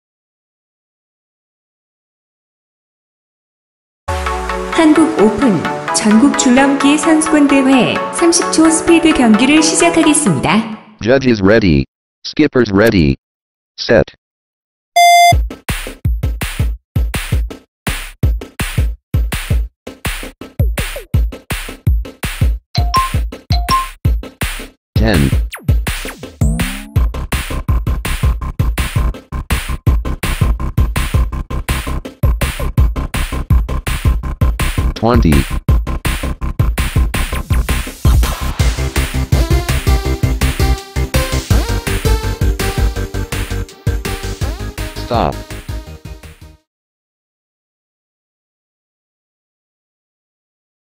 2020 한국오픈 온라인 전국줄넘기 선수권대회 30초 스피드 번갈아 (초등 2학년 박건영) 30sec Speed Jump rope
2020 한국오픈 온라인 전국줄넘기 선수권대회

30초 스피드 번갈아 초등 2학년